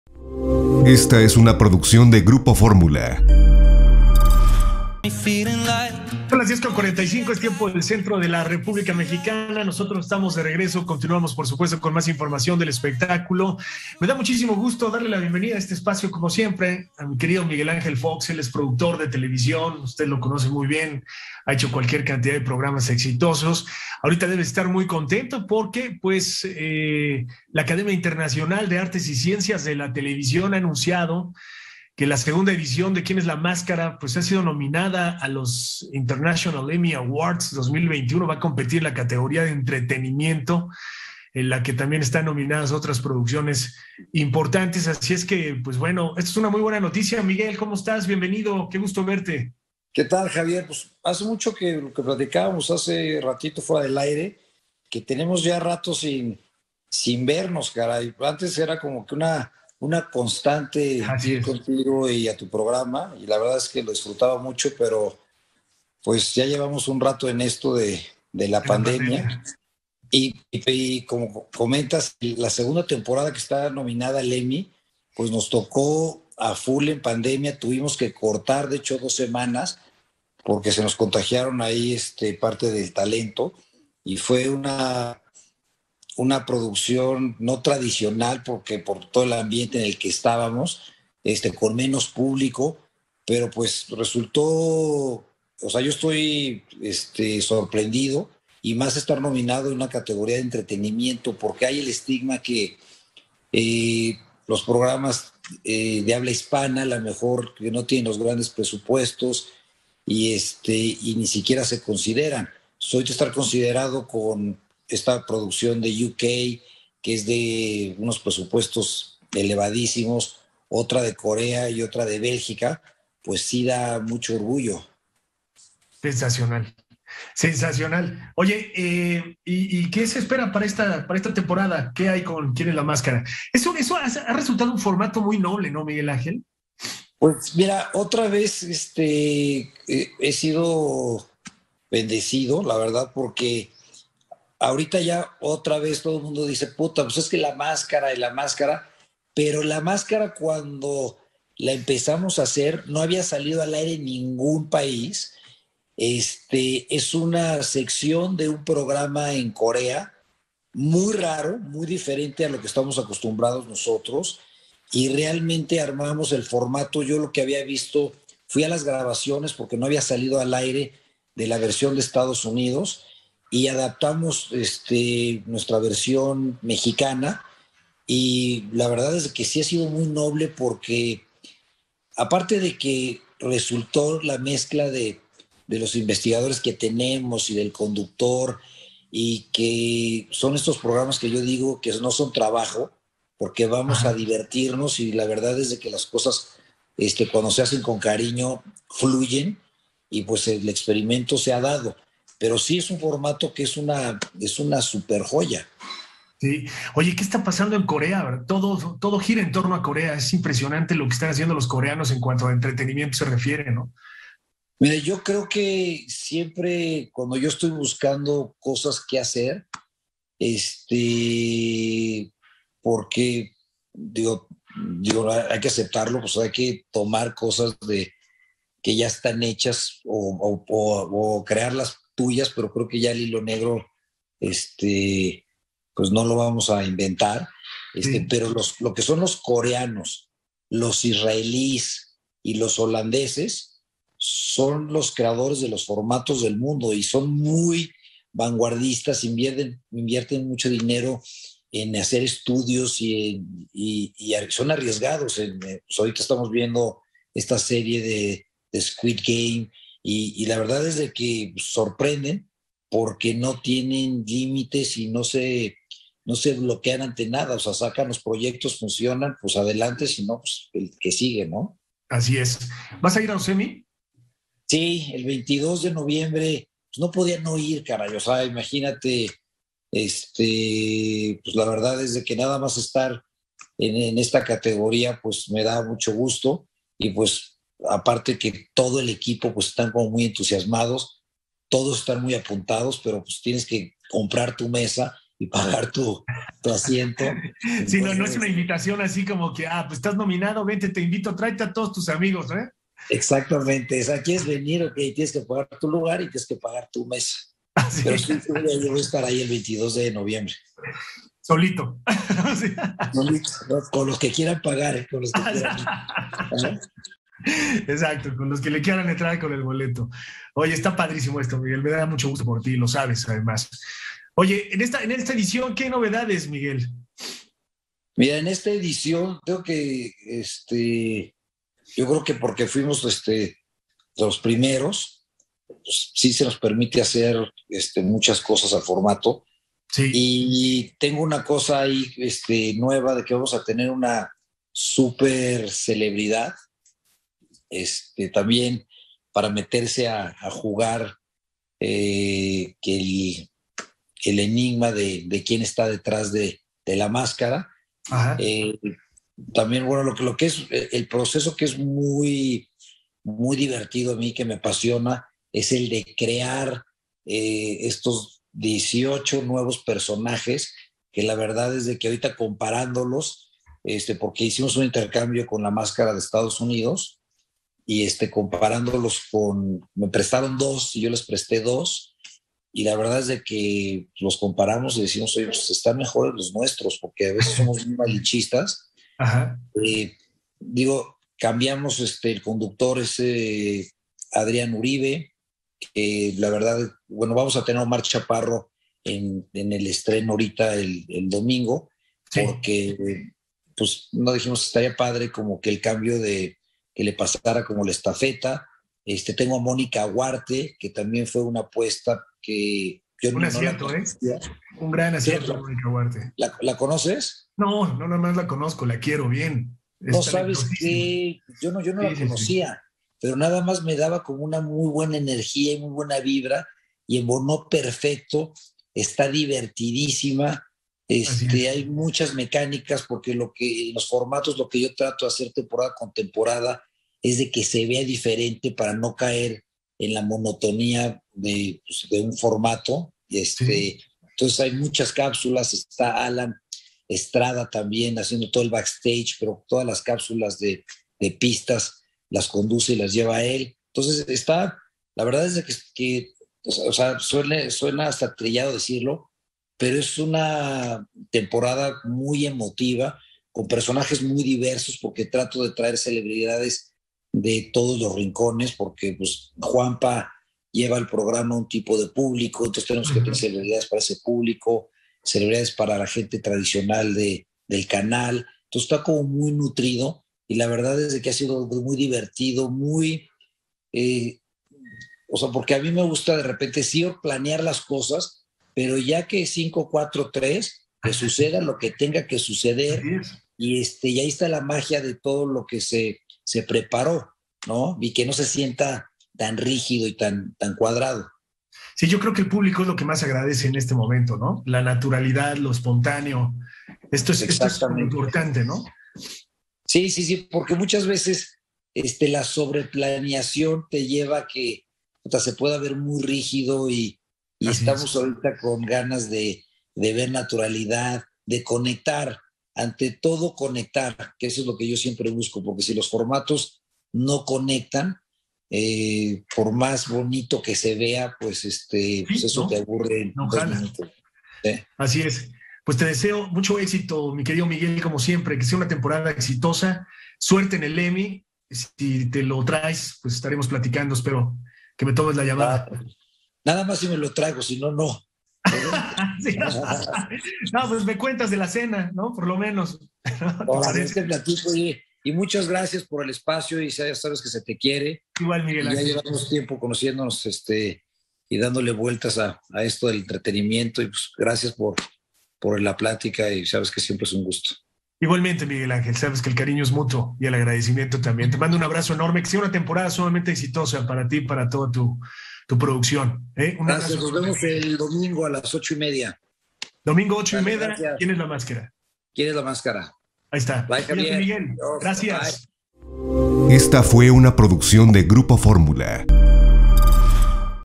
Revelan quienes representarán a ¿Quién es la máscara? en los premios Emmy 2021
Ellos son los famosos que estarán representando a ¿Quién es la máscara? en los premios Emmy 2021.

Grupo Fórmula